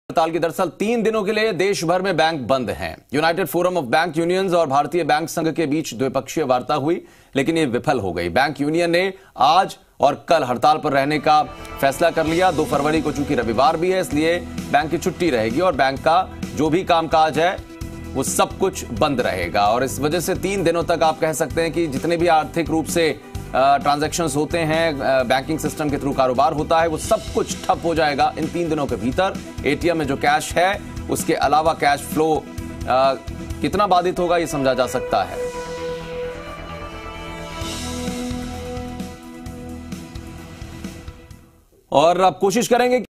बैंकों की दो दिन की देशव्यापी हड़ताल, तीन दिन तक बंद रहेंगे देशभर के बैंक
आज से तीन दिनों के लिए देशभर में बैंक बंद रहेंगे। यूनाइटेड फोरम आफ बैंक यूनियन्स और भारतीय बैंक संघ के बीच हुई द्विपक्षीय वार्ता विफल होने से बैंक यूनियन ने आज और कल हड़ताल पर रहने का फैसला किया है।